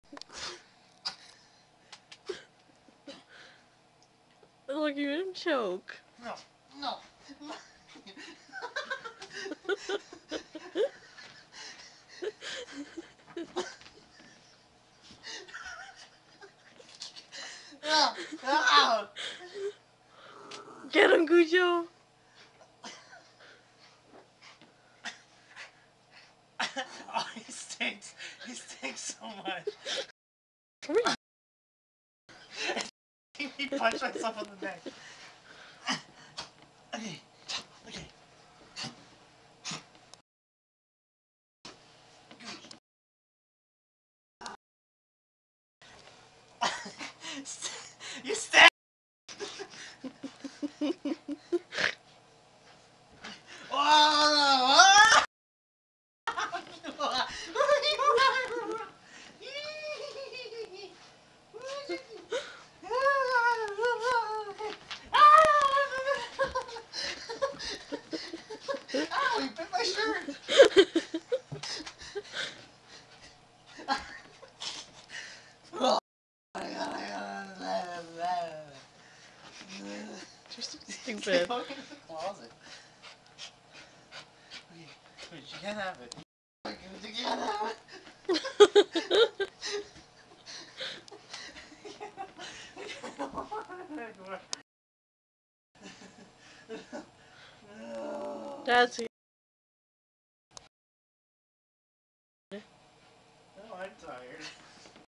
Matt bugging Cujo.
Cujo fighting for his lover.
aka scrap of cloth that once was Dolphin.